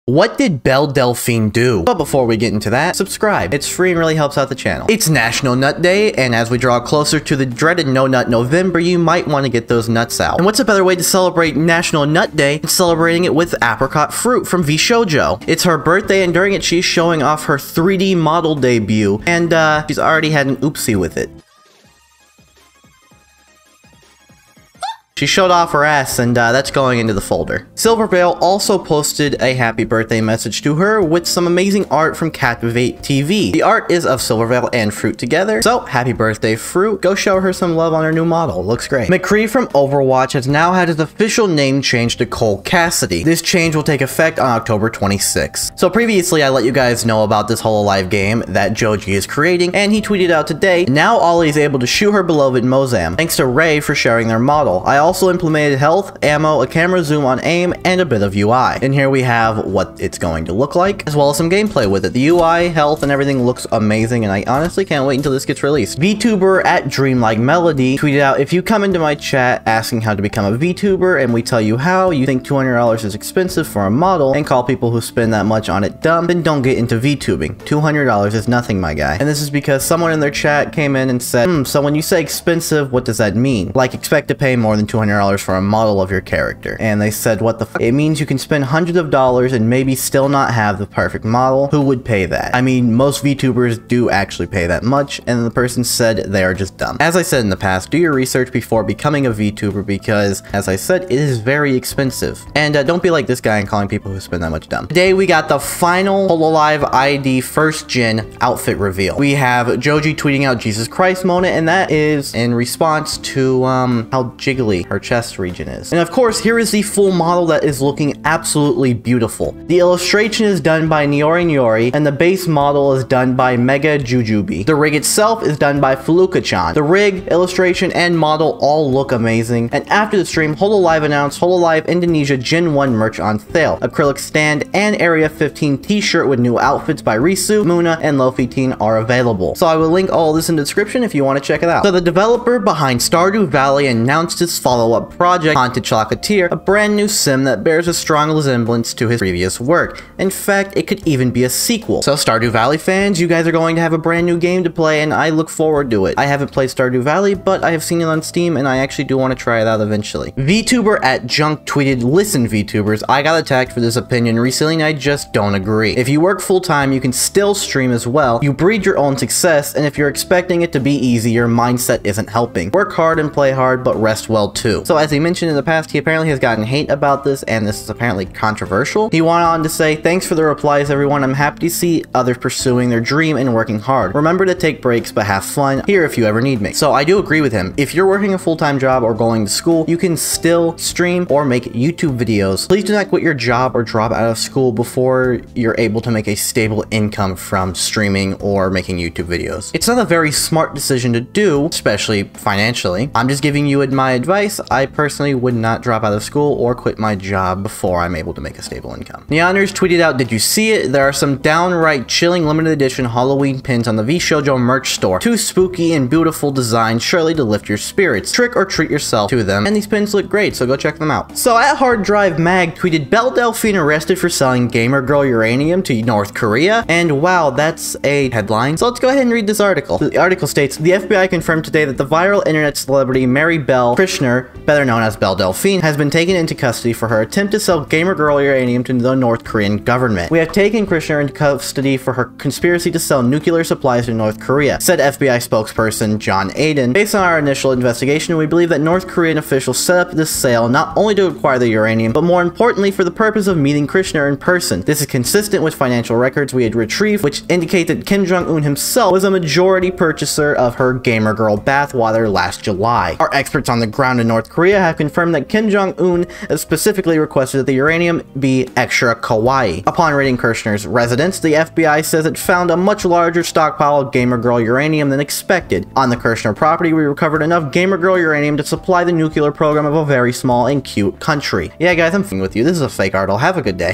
Belle Delphine ARRESTED? | Froot's New Model, Moona New Outfit, Vtubers Getting Hate? Hololive GAME
In this video we talk about Belle Delphine getting arrested, Froot's birthday stream and her new amazing 3D model as well as Moona's new outfit! Vtubers have also been getting some more hate for the price of their models...

If any creators or artists would like their work removed from the thumbnail please reach out and I would be more than happy to remove it!

Timestamps:
Intro(0:00)
National Nut Day: (0:05)
Apricot(Froot)'s Birthday and Model: (0:10)
McCree Name Change: (0:39)
Hololive Game: (0:49)
Vtuber Price Hate: (1:06)
Moona Outfit Reveal: (1:45)
Stardew Valley's Creator new game: (2:22)
Streaming advice: (2:40)
Nyanners Pins: (3:29)
BELLE DELPHINE ARRESTED?!: (3:41)

A virtual YouTuber or VTuber is an online entertainer who uses a virtual avatar generated using computer graphics and motion capture software or technology.